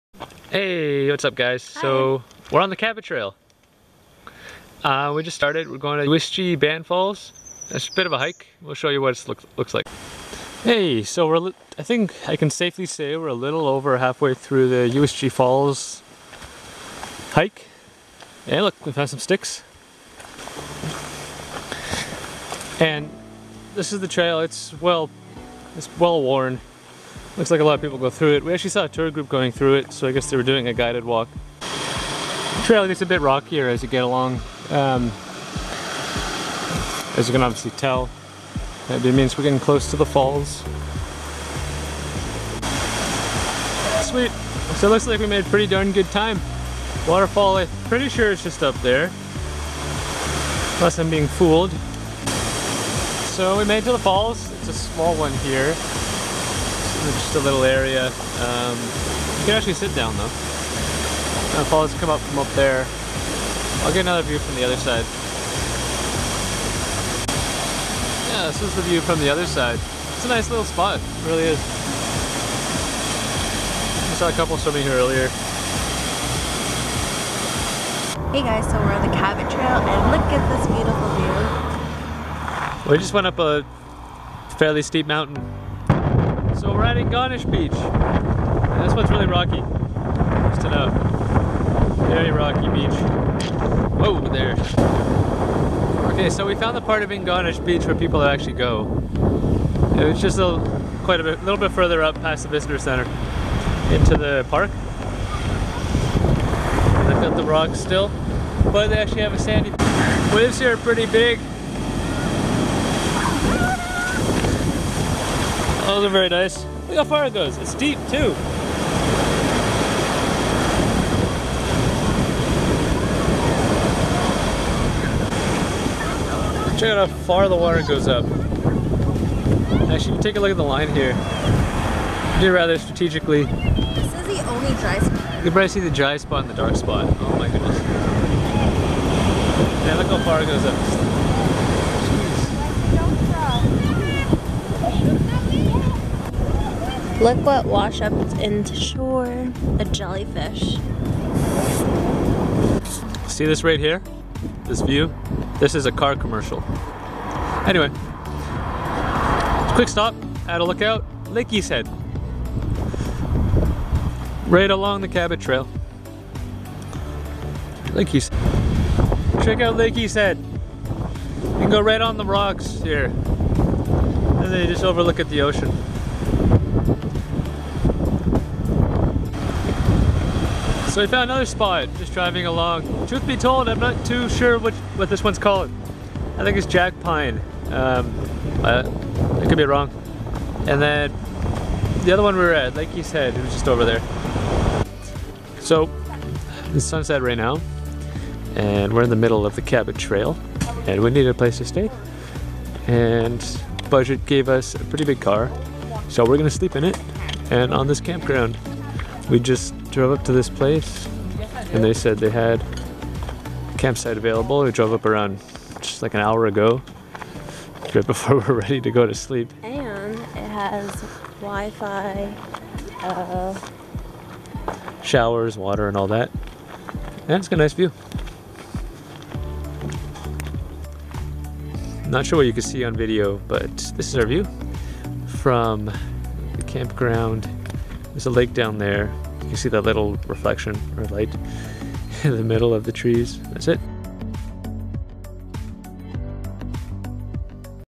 Day 1 Cabot Trail and Uisge Ban Falls - Canada Vlog - Jessie and Luke
We both the had opportunity to explore our own country because as a part of Canada's 150th birthday, Via Rail offered a summer unlimited pass that let us travel across the country by train for the whole month of July. We decided to document our trips to keep as a memory for us and for our friends and family. Thank you for visiting our channel!

~ AIRBNB: Use my link to get $40 off your first Airbnb stay.